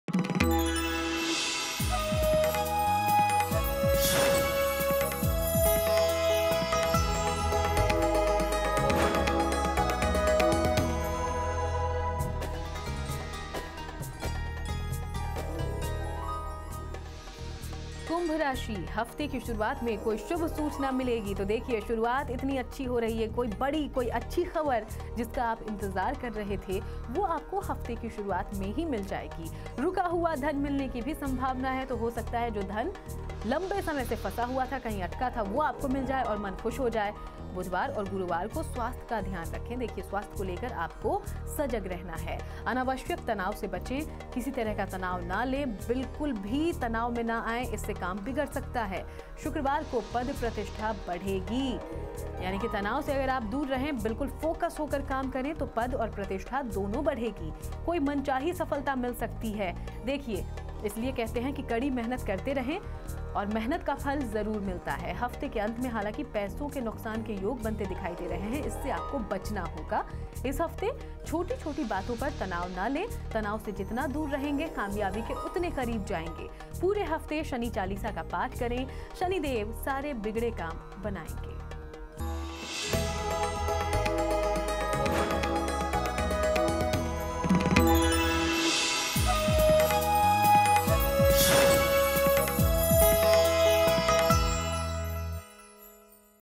कुंभ राशि का साप्ताहिक राशिफल | 11th Sept - 17th Sept 2017 | Shailendra Pandey | Astro Tak
कुंभ राशि का साप्ताहिक राशिफल- 11 सितंबर से 17 सितंबर के बीच क्या होने वाला है कुंभ राशि के लोगों के जीवन में खास और किन बातों का रखना होगा विशेष ध्यान, जानिए कुंभ राशि वालों के लिए  शैलेंद्र पांडेय की साप्ताहिक भविष्यवाणी